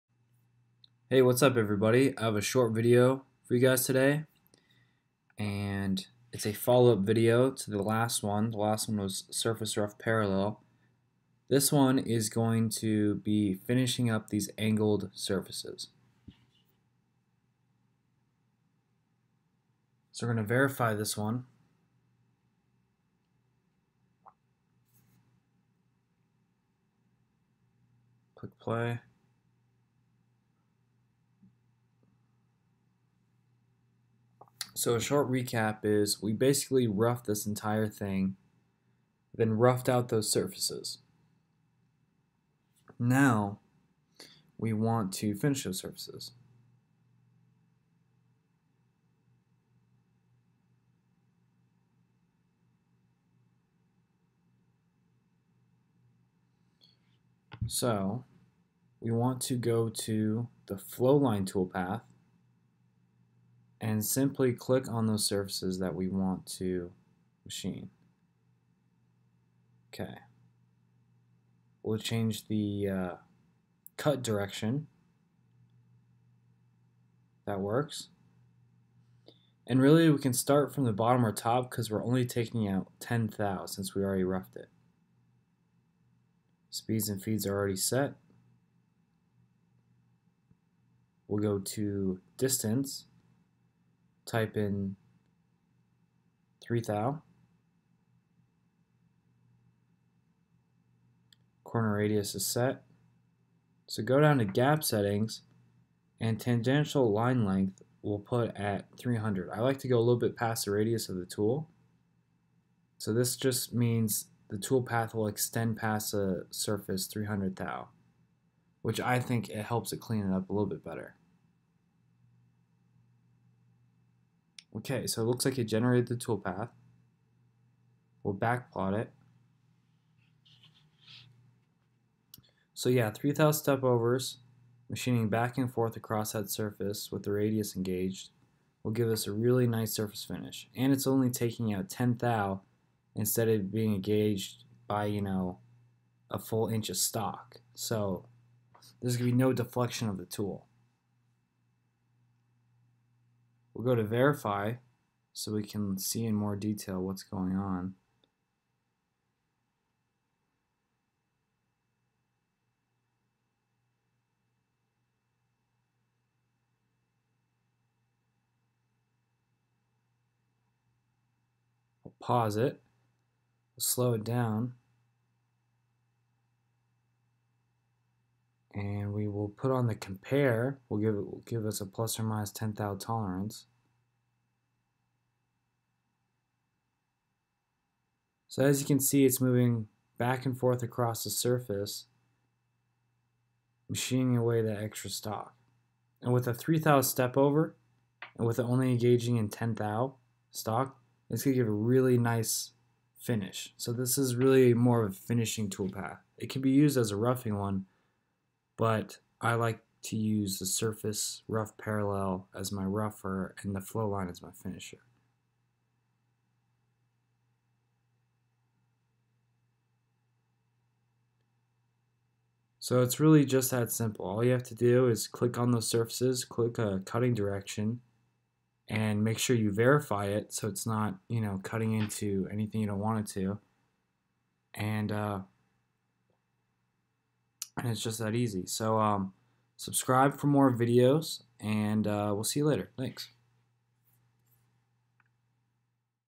Flowline - Mastercam
Follow up video on what tool path to use after surface rough parallel.

Make Chips Solve Problems